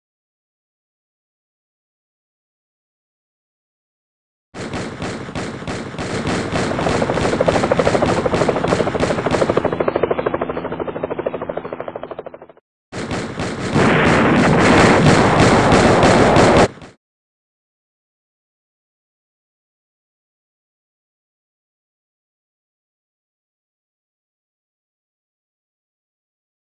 World War 1
An animated short film created during Stopmotion Animation Camp at the Ilsley Public Library.